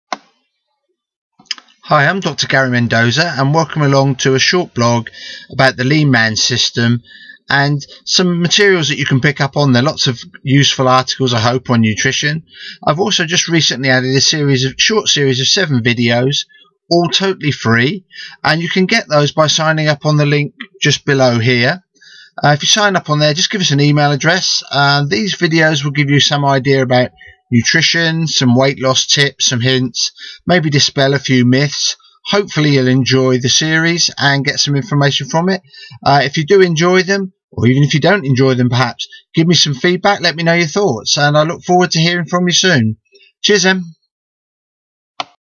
LEAN Man System - The Magnificent Seven Videos
View A brief introduction to the free video series and their content: Weight loss and nutrition hints and tips.